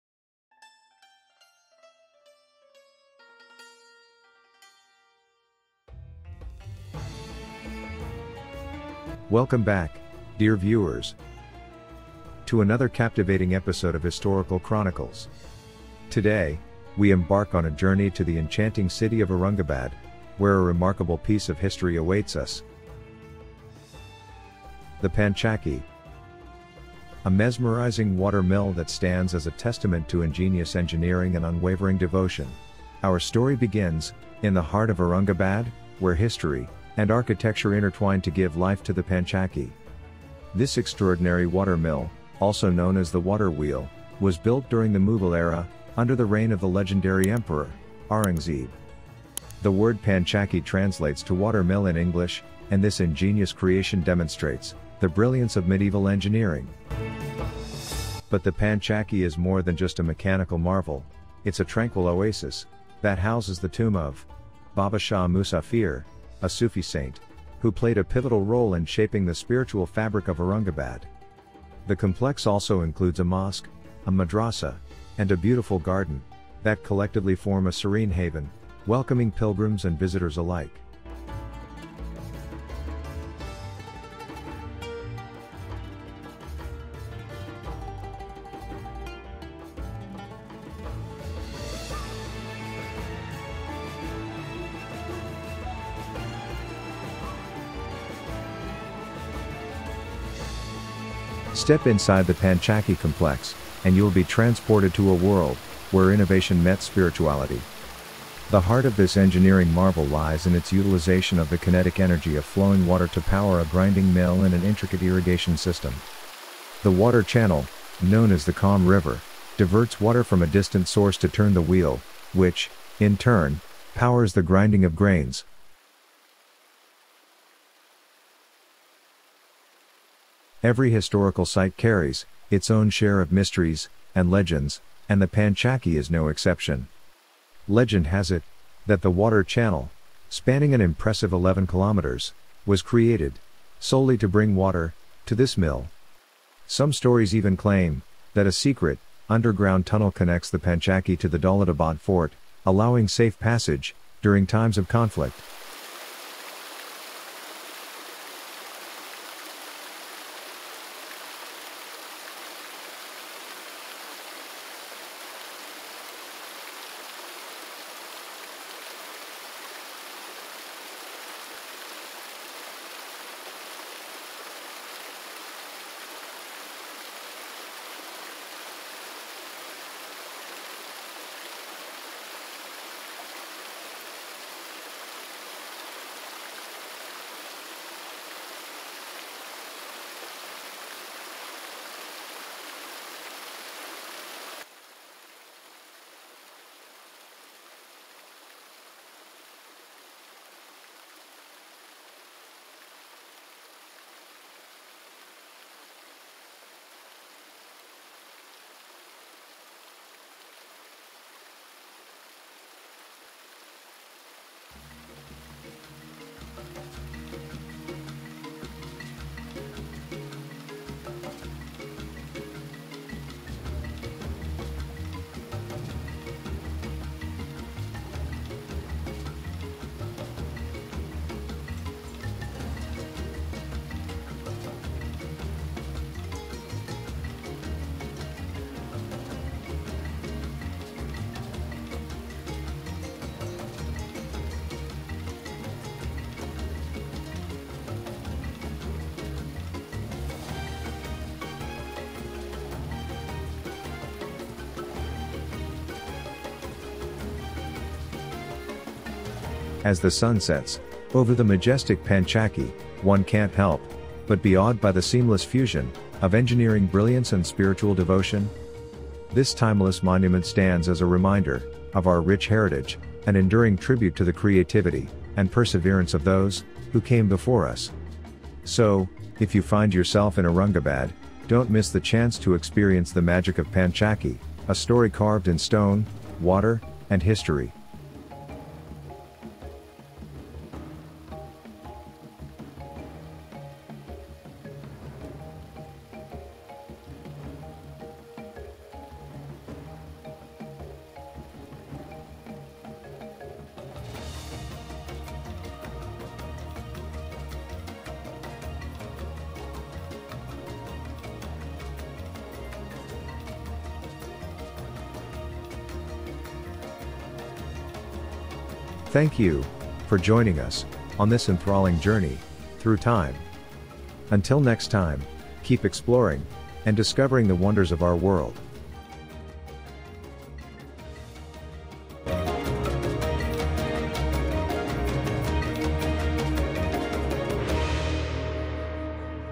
The Engineering Marvel of Mughal Era Revealed!
🏰 Explore the Enchanting Panchakki, the water mill of Aurangabad 🏰
Immerse yourself in the captivating history of Panchakki,a fascinating tale, the awe-inspiring water mill nestled in the heart of Aurangabad. Join us on a virtual journey through this remarkable architectural marvel that seamlessly blends ingenious engineering with spiritual serenity. Discover the secrets behind its intricate water wheel mechanism, the legends that shroud its existence, and the tranquil oasis it offers to pilgrims and wanderers alike. Don't miss out on experiencing the magic of Panchakki – a testament to the brilliance of the Mughal era. 🕊️🌊🕌